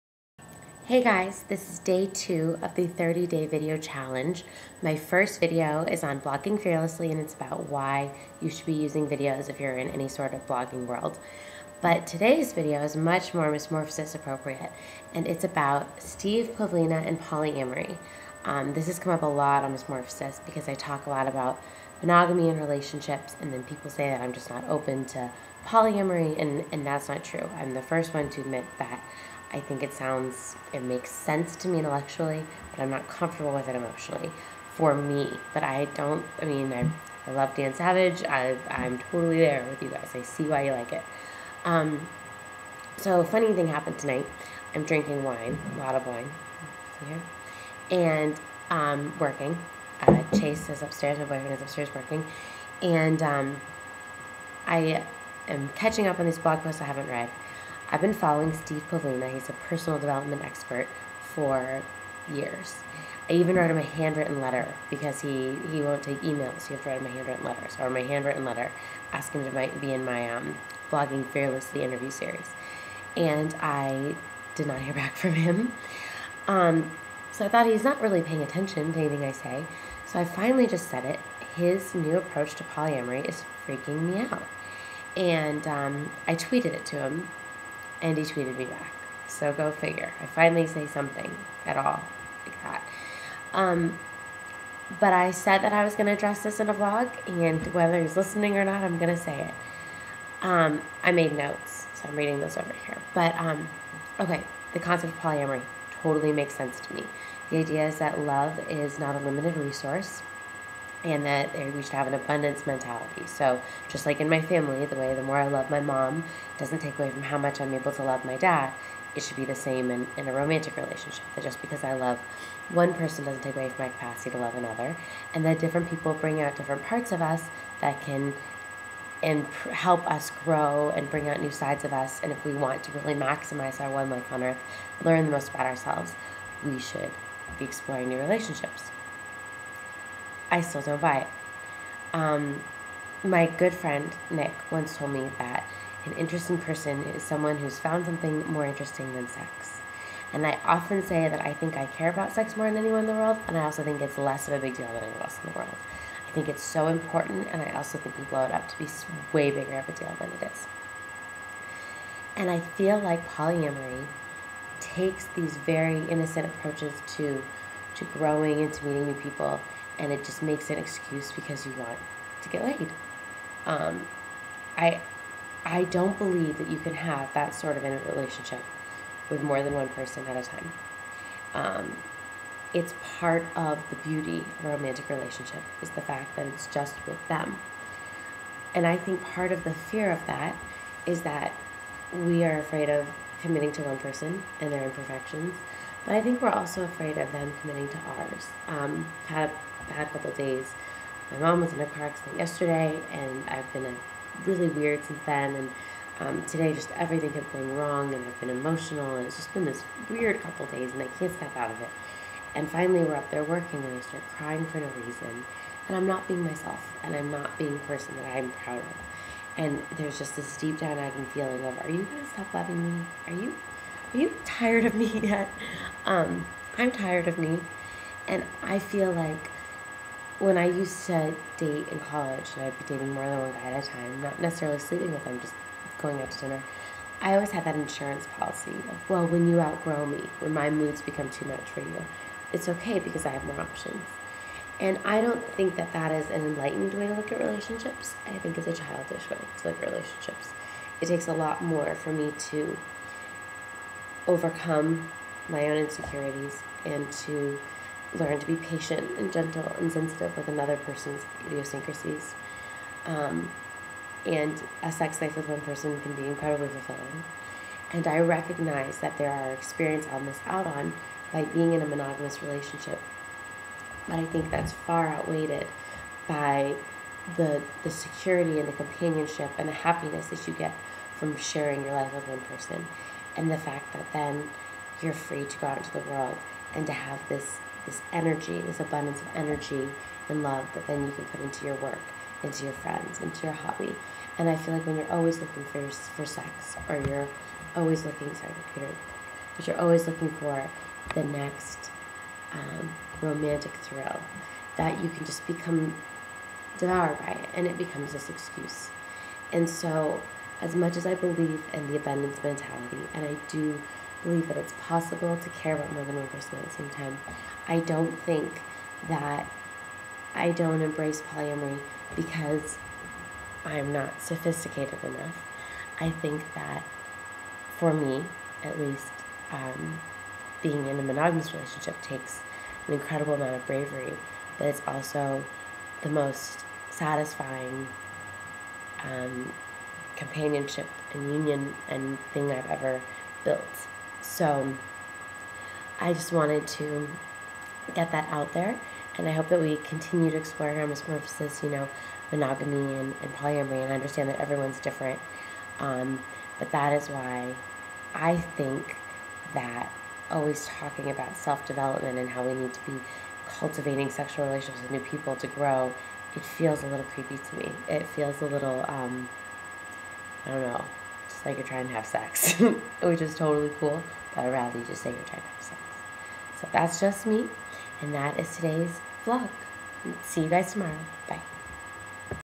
I'm Still Not Down with Polyamory - 30 Day Vlog Challenge - Jen Gargotto
I was first told I didn't understand polyamory when I posted this

The discussion goes on. Now I talk to Steve Pavlina about why although I usually love his stuff, I'm not buying the polyamory stuff.